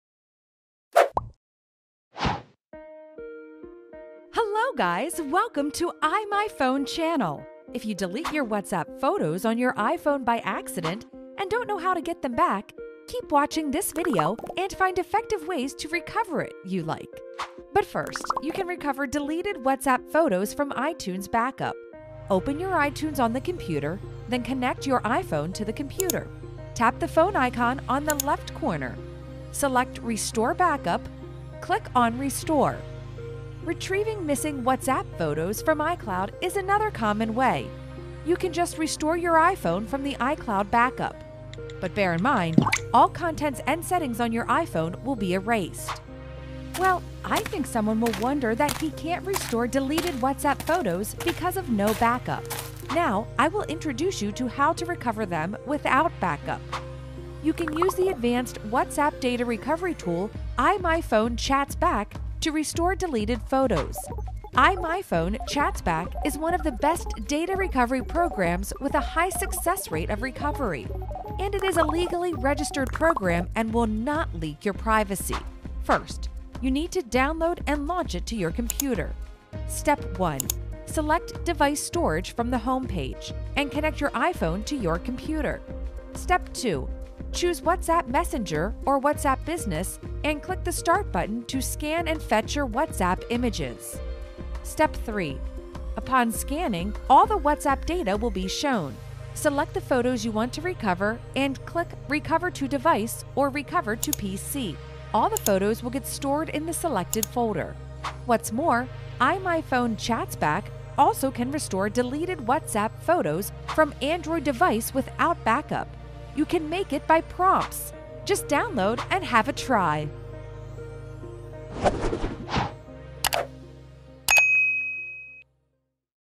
How to Recover Deleted WhatsApp Photos on iPhone
Learn how to get back your deleted WhatsApp photos on your iPhone!
How do you get back your WhatsApp photos on your iPhone once they are deleted permanently? Here’s the solution! In this video, we’ll show you how to recover deleted WhatsApp photos/videos on iPhone without backup or from iTunes/iCloud backup. Here is the article
Timestamp:
00:19 Recover WhatsApp Photos from iTunes Backup
00:33 Recover WhatsApp Photos from iCloud
00:53 Restore WhatsApp Photos without Backup